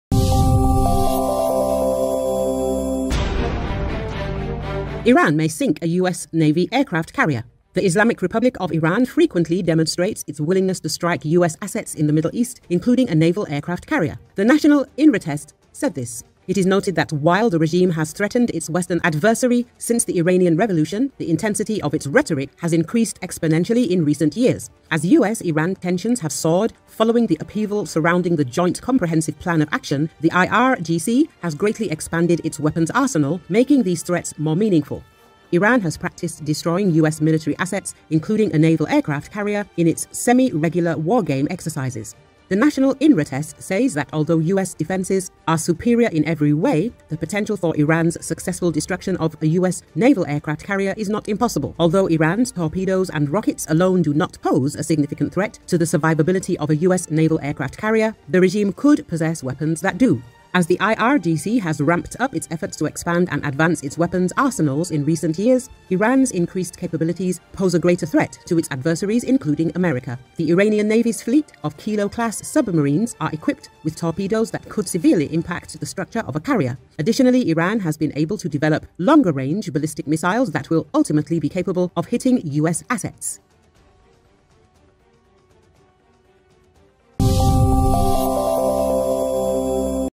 Iran may sink a U.S. Navy Aircraft Carrier
The Islamic Republic of Iran frequently demonstrates its willingness to strike U.S. assets in the Middle East, including a naval aircraft carrier. The National İnretest said this.
İt is noted that While the regime has threatened its Western adversary since the Iranian Revolution, the intensity of its rhetoric has increased exponentially in recent years. As U.S.-Iran tensions have soared following the upheaval surrounding the Joint Comprehensive Plan of Action, the IRGC has greatly expanded its weapons arsenal, making these threats more meaningful. Iran has practiced destroying U.S. military assets, including a naval aircraft carrier, in its semi-regular wargame exercises.
The National İnretest  says that although U.S. defenses are superior in every way, the potential for Iran’s successful destruction of a U.S. naval aircraft carrier is not impossible. Although Iran’s torpedoes and rockets alone do not pose a significant threat to the survivability of a U.S. naval aircraft carrier, the regime could possess weapons that do.
As the IRGC has ramped up its efforts to expand and advance its weapons arsenals in recent years, Iran’s increased capabilities pose a greater threat to its adversaries, including America. The Iranian Navy’s fleet of Kilo-class submarines are equipped with torpedoes that could severely impact the structure of a carrier.
Additionally, Iran has been able to develop longer-range ballistic missiles that will ultimately be capable of hitting U.S. assets.